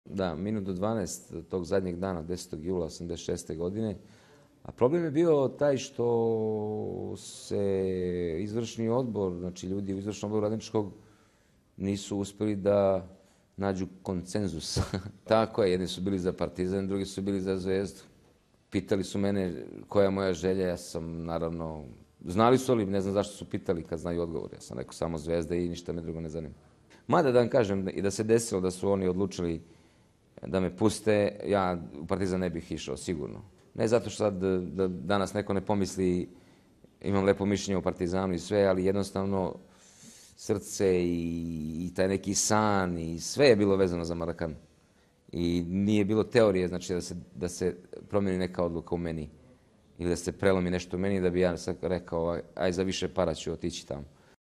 Poštujem Partizan, ali je srce uvek vuklo ka Marakani - Dragan Stojković - Mojih TOP 11
Svake srede u 21h na Mojih TOP 11 sa Nebojšom Petrovićem YT kanalu čeka vas nova epizoda!

✅ Autor emisije : Nebojša Petrović